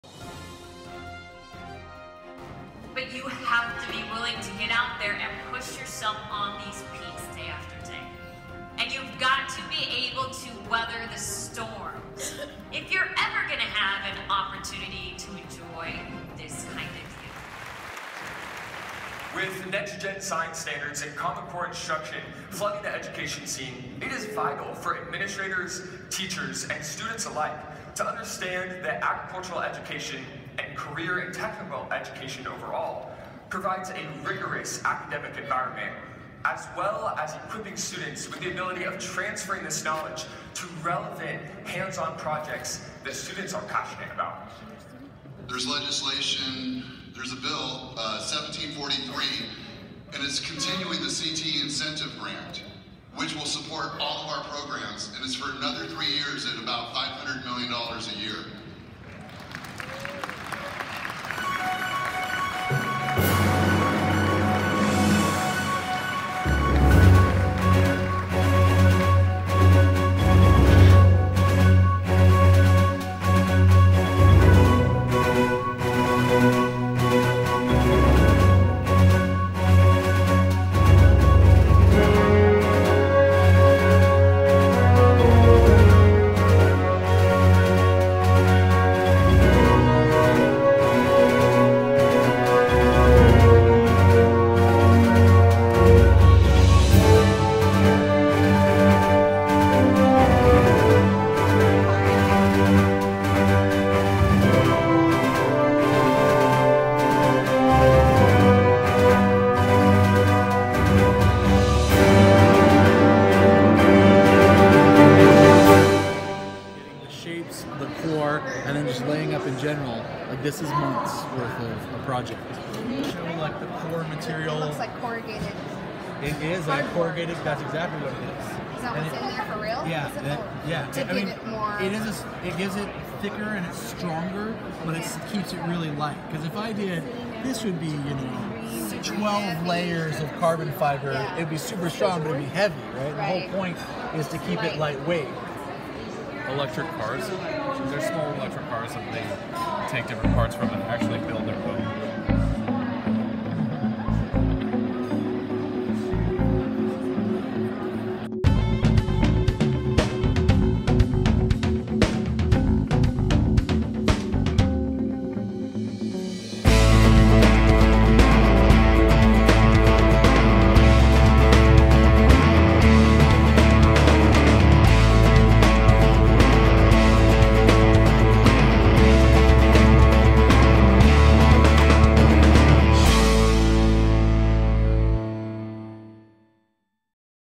Educating for Careers
March 4-6, 2018, annual conference in Sacramento. California Partnership Academies. Keynotes speakers, intensive seminars, discussions, awesome presentation by teachers and students.